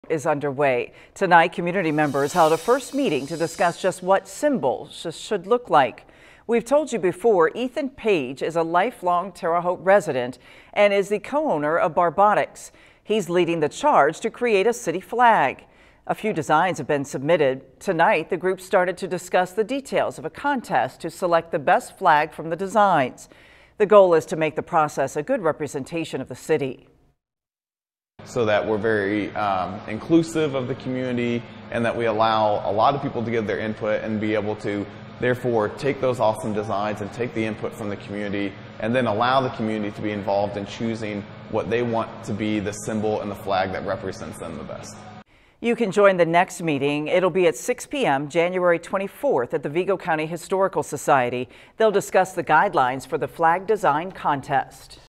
Group continues Terre Haute flag discussion
The idea to create a flag for Terre Haute is underway.